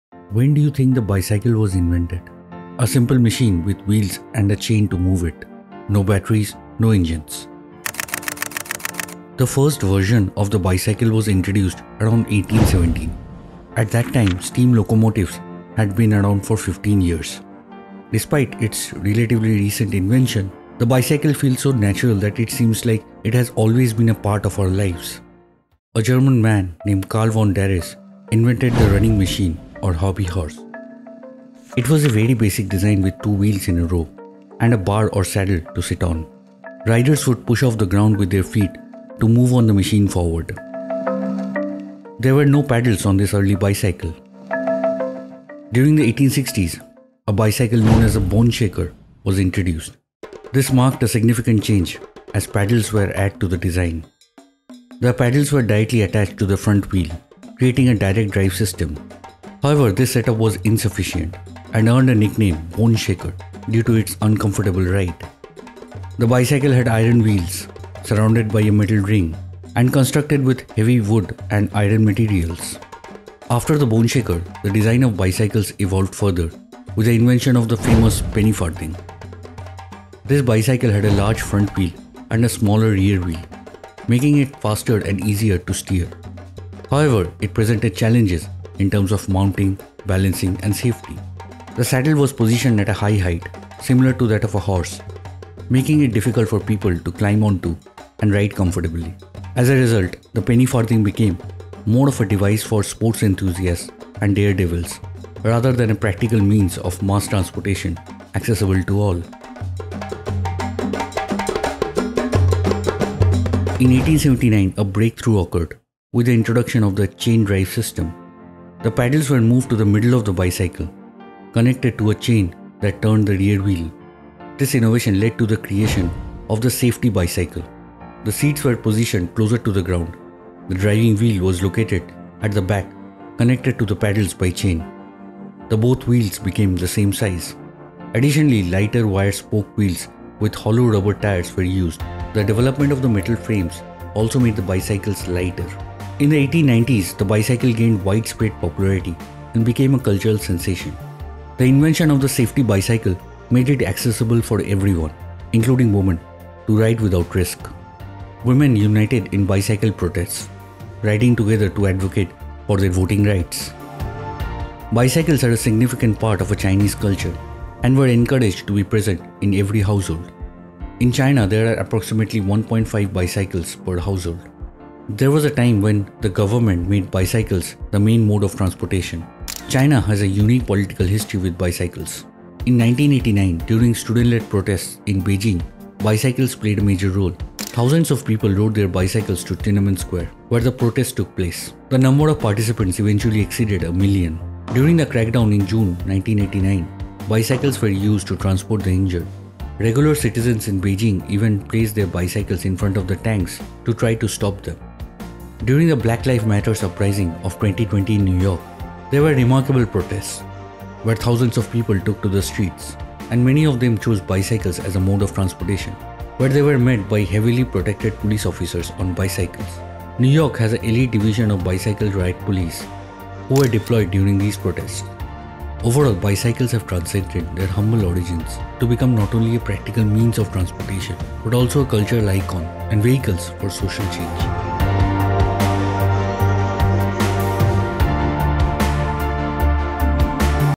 How has the BICYCLE changed over time
Explore the evolution of the bicycle, a simple machine born in 1817 without engines or batteries. Initially a "running machine" without pedals, it transformed into the boneshaker, Penny-farthing, and safety bicycle. In 1879, the chain drive system revolutionized cycling, leading to the safety bicycle's birth. Bicycles gained popularity in the 1890s, empowering women and even influencing political movements in China and protests in New York. From Tiananmen Square to the Black Lives Matter uprising, bicycles became symbols of social change, proving that these two-wheeled wonders are more than just a mode of transport.